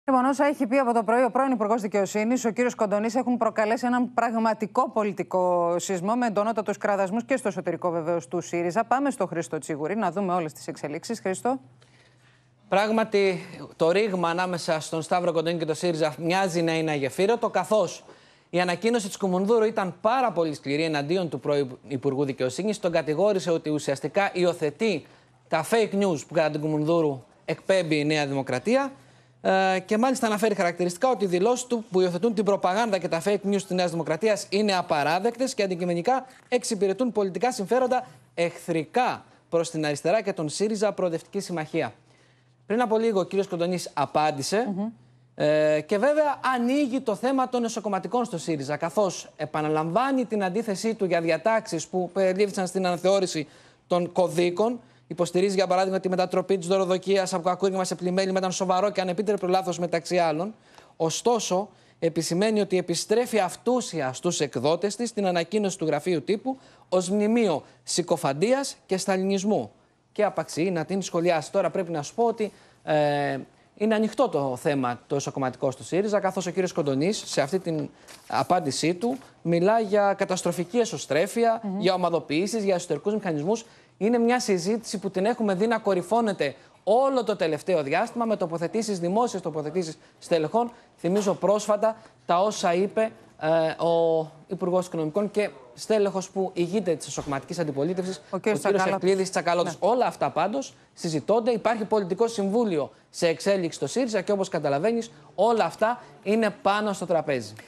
Θύελλα στον ΣΥΡΙΖΑ για τον Κοντονή | Κεντρικό δελτίο ειδήσεων 08/10/2020 | OPEN TV
Όσα έχει πει από το πρωί ο πρώην υπουργός Δικαιοσύνης, ο κ . Κοντονής έχουν προκαλέσει ένα πραγματικό πολιτικό «σεισμό», με εντονότατους κραδασμούς και στο εσωτερικό του ΣΥΡΙΖΑ.